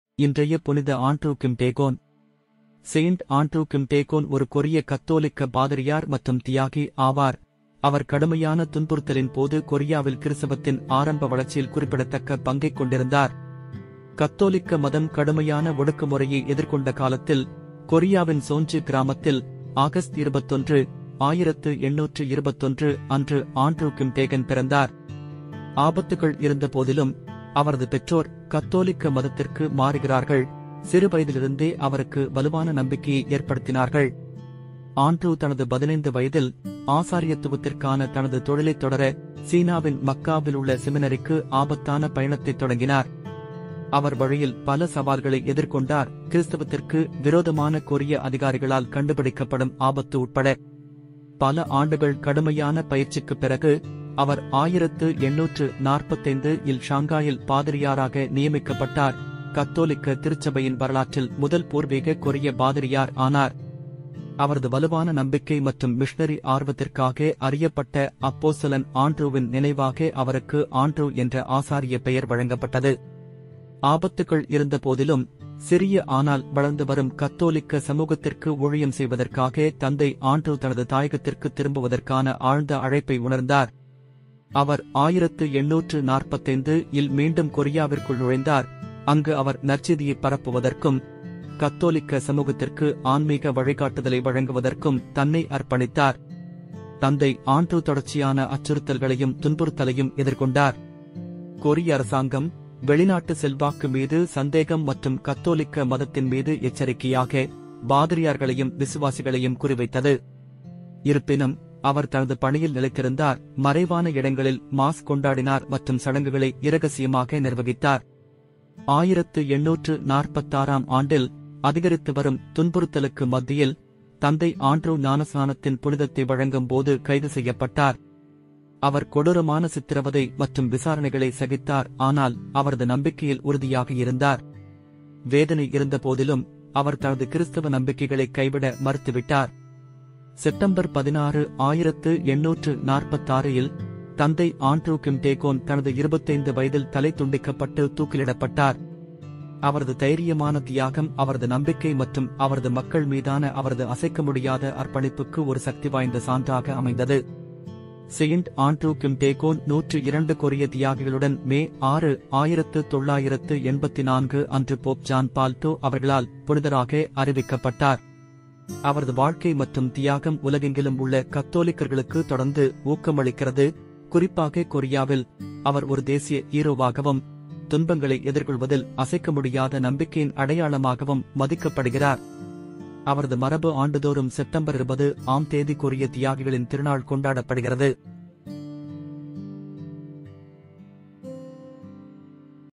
இன்றைய புனித ஆண்ட்ரூ கிம் டேகோன் - செப் 20
எல்லோருக்கும் வணக்கம்!
இன்று புனித ஆண்ட்ரூ கிம் டேகோன்பற்றிய வாழ்க்கைச் சம்பவங்கள் மற்றும் சுருக்கத்தைப் பார்ப்போம்.
இவரது விழா
செப்டம்பர் 20ம் தேதி கொண்டாடப்படுகிறது